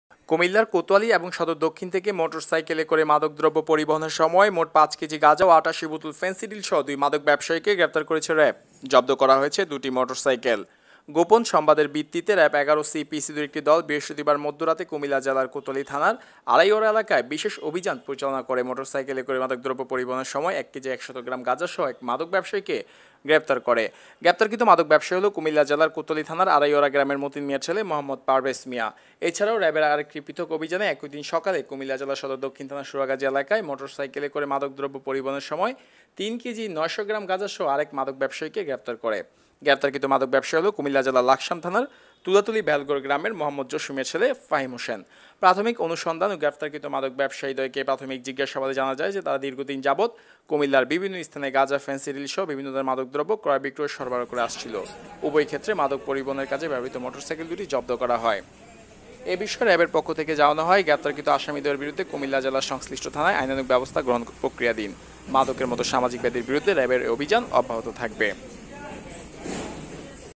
কুমিল্লায় র‌্যাবের হাতে ইয়াবা ফেন্সিডিল পাচারকালে দুই মাদক ব্যাবসায়ী গ্রেফতার।
আপডেটঃ ১২/০৮/২০২১
এনসিবি রিপোর্ট ।।
কুমিল্লার কোতয়ালি এবং সদর দক্ষিণ থেকে মোটর সাইকেলে করে মাদকদ্রব্য পরিবহনের সময় মোট ০৫ কেজি গাঁজা ও ৮৮ বোতল ফেন্সিডিলসহ দুইজন মাদক ব্যবসায়ী গ্রেফতার। দুইটি মোটর সাইকেল জব্দ।